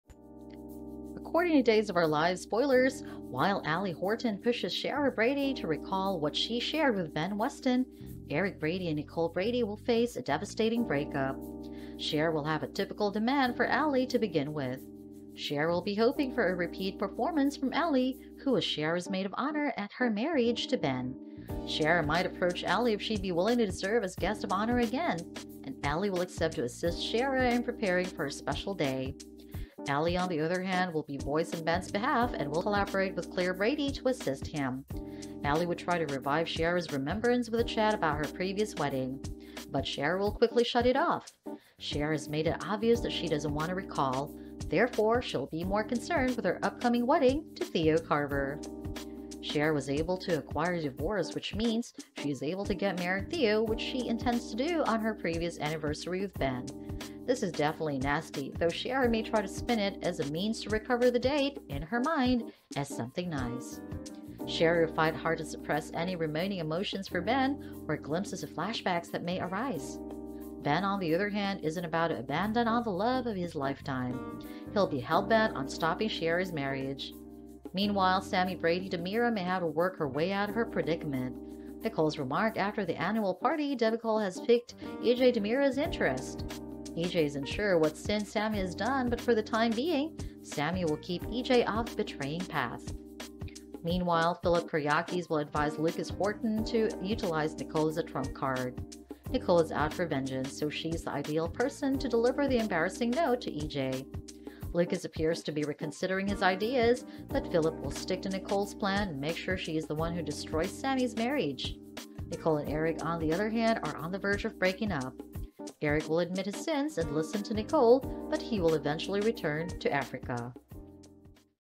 Days of Our Lives Spoilers: Nicole and Eric Shocking Breakup, Allie Persuades Ciara Remember Ben
According to Days of Our Lives Spoilers, Tuesday, July 20, while Allie Horton pushes Ciara Brady to recall what she shared with Ben Weston, Eric Brady and Nicole Brady will face a devastating breakup. Ciara will have a typical demand for Allie on this episode.

Thanks for coming by our channel!
We deliver Days of Our Lives Cast Updates & Spoilers here!